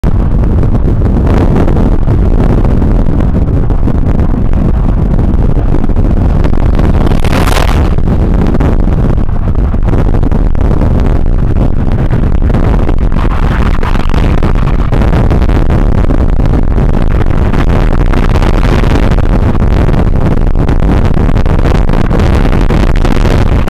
High Performance Police Cars Responding.
Four Central Motorway Police Group  Police Units Responding to a Unknown Incident.

Apologies for the sound.